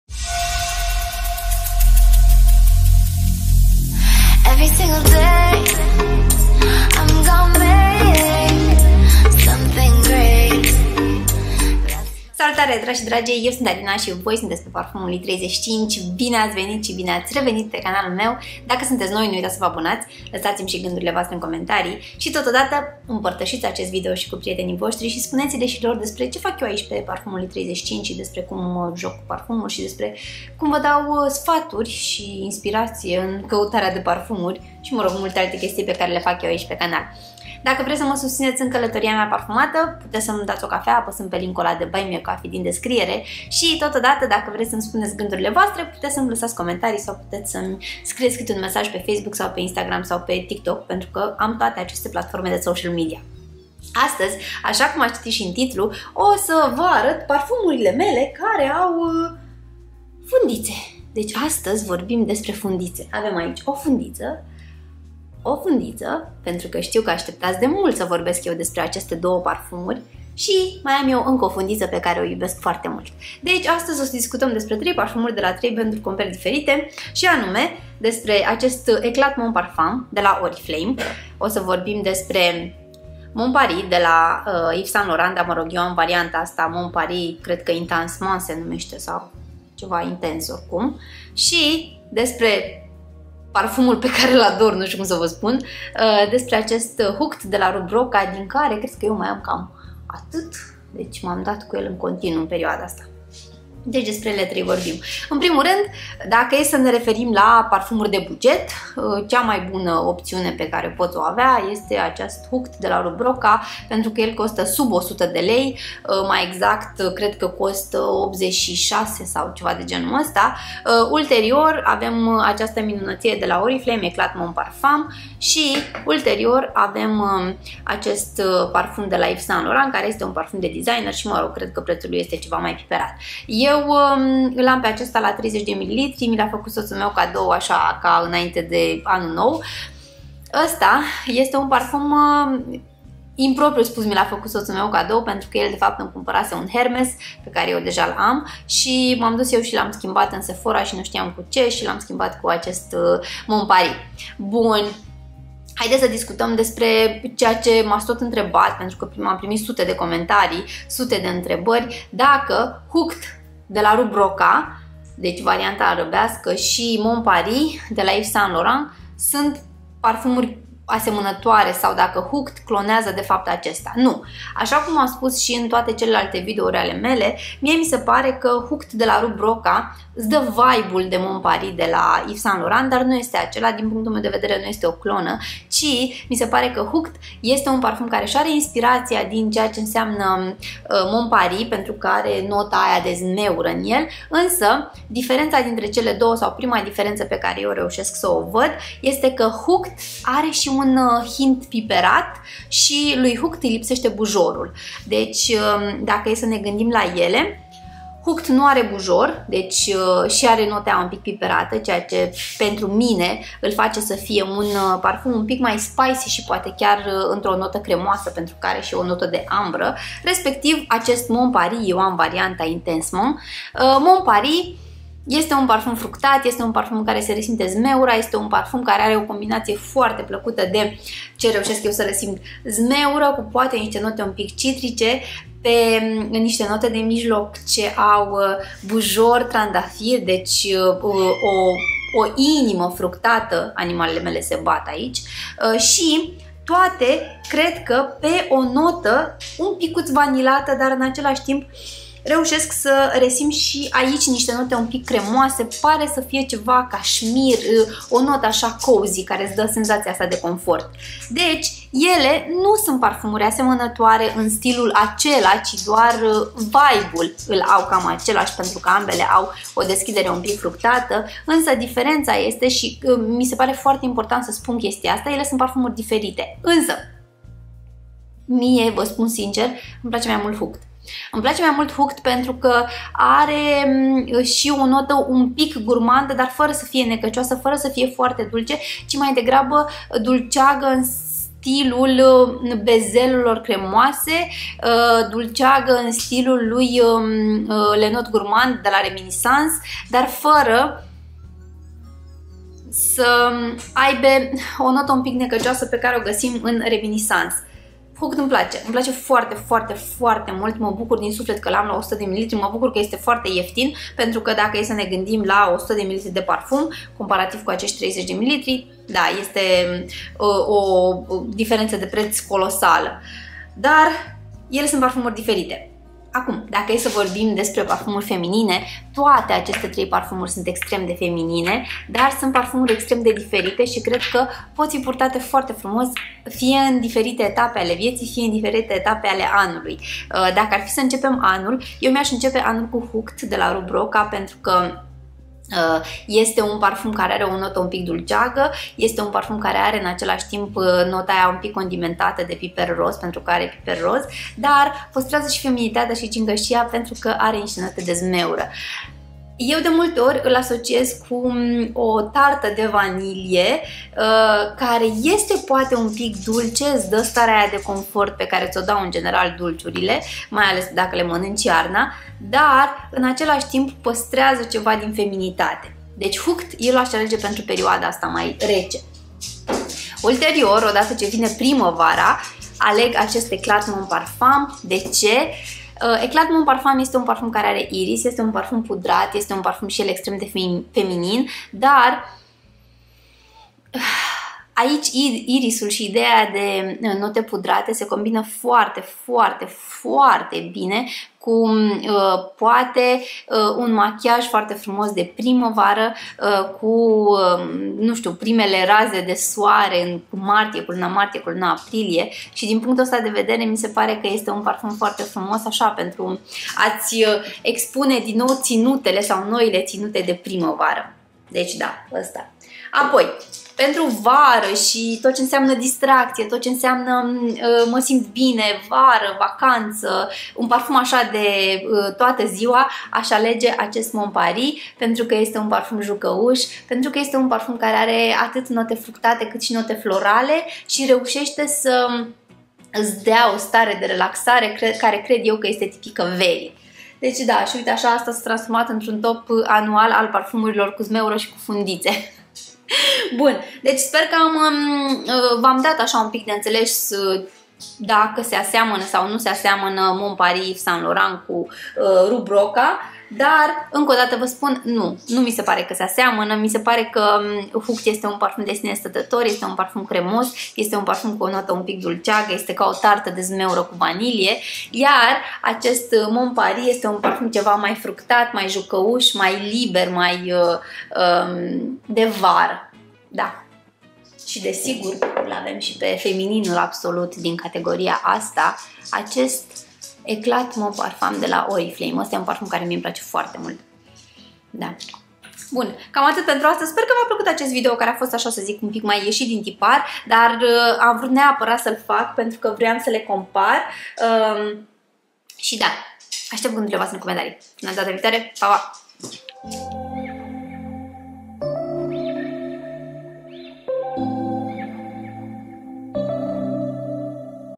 YSL Mon Paris vs. Hooked - Rue Broca. Care Parfum ți se Potrivește? Sunt alternative?
De unde imi cumpar parfumurile:
ww.Parfumas.ro - daca vreau Afnan, Al Haramain, , Al Rehab, Orientica si altele